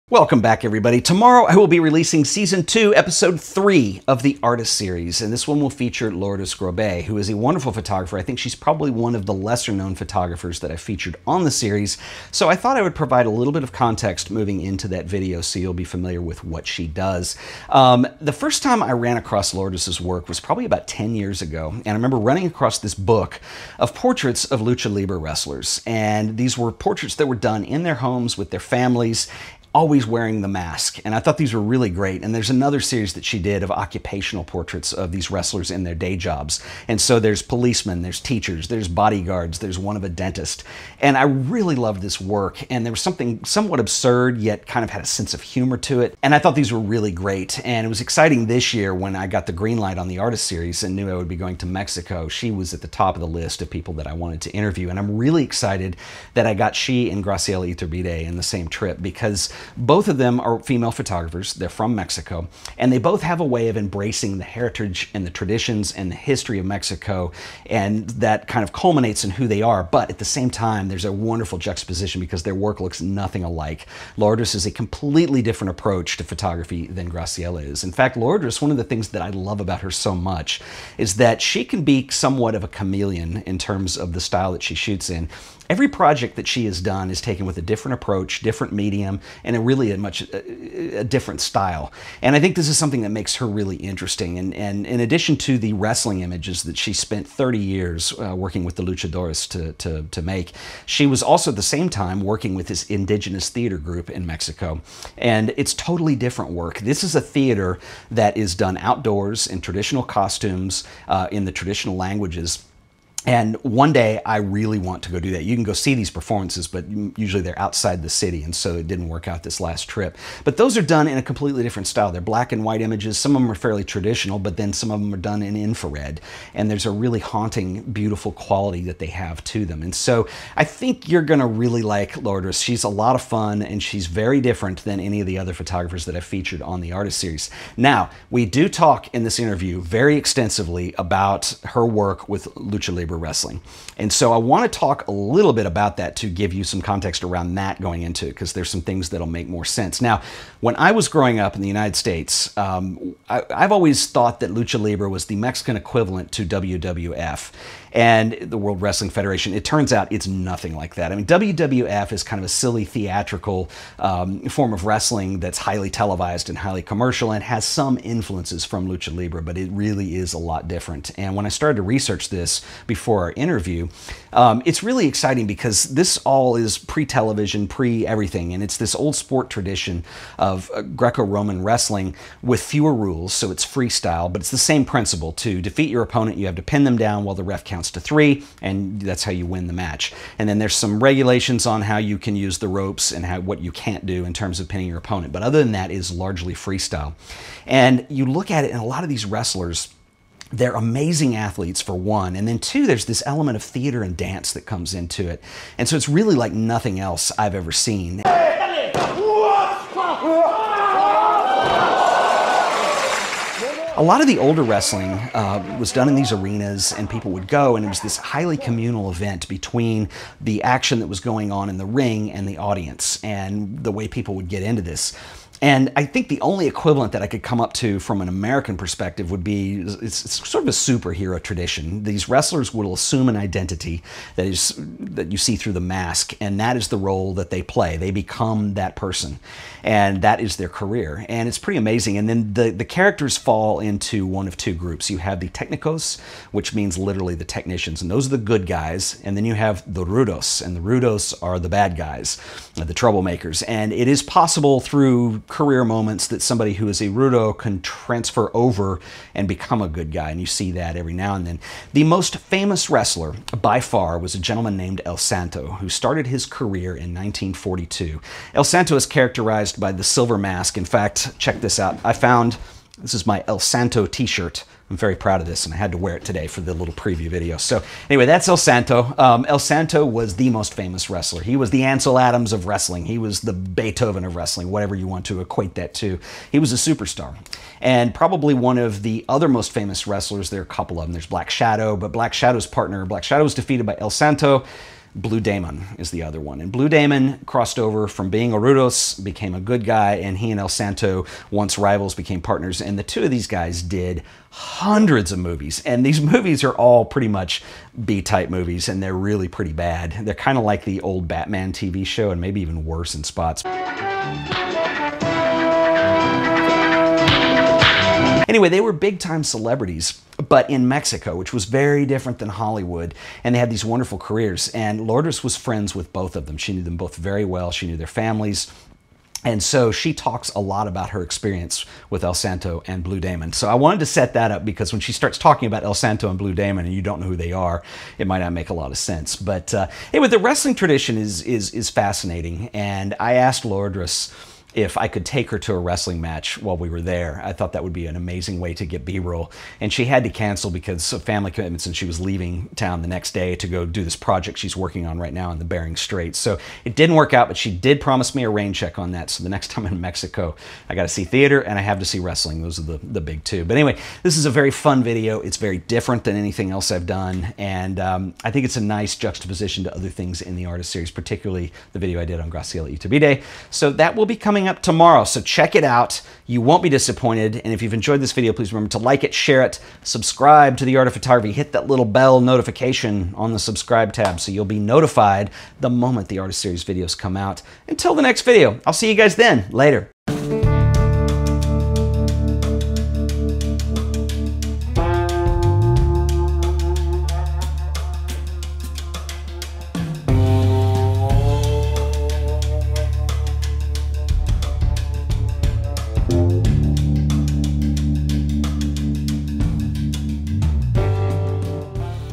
Lucha Libre Photography?
Tomorrow is the release of S2E3 of the Artist Series with features Lourdes Grobet. Lourdes is a brilliant photographer known best for her work with Lucha Libre which was a photography project she worked on for 30 years. She worked closely photographing famous luchadores such as El Santo and Blue Demon over her career.

I want to provide some context of her photography as a lead in to her Artist Series film.

Ted Forbes
The Art of Photography
USA

I make videos covering famous photographers, photography techniques, composition, the history of photography, philosophy and much more.